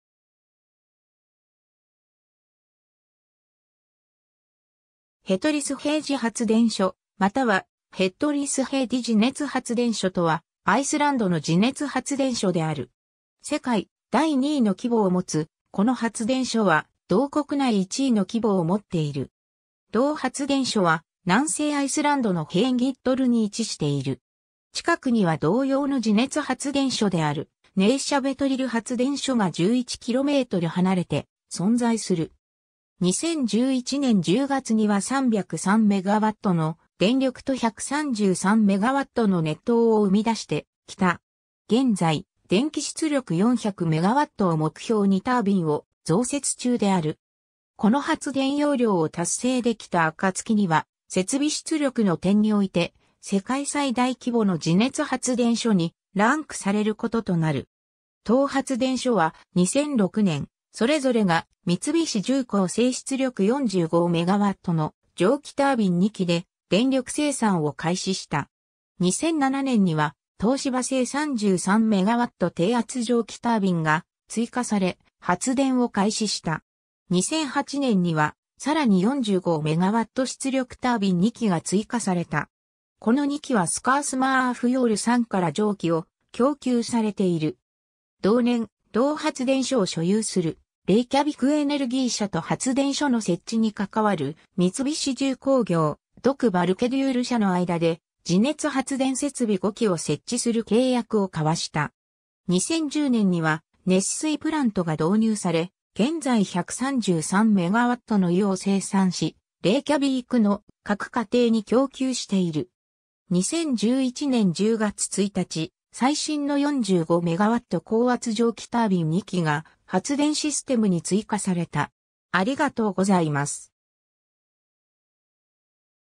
ヘトリスヘイジ発電所
ヘトリスヘイジ発電所またはヘッドリスヘイディ地熱発電所（ヘットリスヘイディ地熱発電所とも。氷島語：Hellisheiðarvirkjun、英：Hellisheiði Power Station）とは、アイスランドの地熱発電所である。
世界第2位の規模をもつこの発電所は、同国内1位の規模をもっている。
同発電所は南西アイスランドのヘインギットルに位置している。
近くには同様の地熱発電所であるネーシャヴェトリル発電所が11 km (7 mi)離れて存在する。
2011年10月には303メガワットの電力と133メガワットの熱湯を生み出してきた。
現在、電気出力400メガワットを目標にタービンを増設中である。
この発電容量を達成できた暁には、設備出力の点において世界最大規模の地熱発電所にランクされることとなる。
 当発電所は、2006年、それぞれが三菱重工製出力45メガワットの蒸気タービン2基で電力生産を開始した。
2007年には、東芝製33メガワット低圧蒸気タービンが追加され、発電を開始した。
2008年には、さらに45メガワット出力タービン2基が追加された。
この2機はSkarðsmýrarfjall山から蒸気を供給されている。
同年、同発電所を所有するレイキャビク・エネルギー社と発電所の設置にかかわる三菱重工業、独バルケデュール社（Balcke－Durr GmbH）の間で地熱発電設備5基を設置する契約を交わした。
 2010年には熱水プラントが導入され、現在133メガワットの湯を生産し、レイキャヴィークの各家庭に供給している。
2011年10月1日、最新の45メガワット高圧蒸気タービン2基が発電システムに追加された。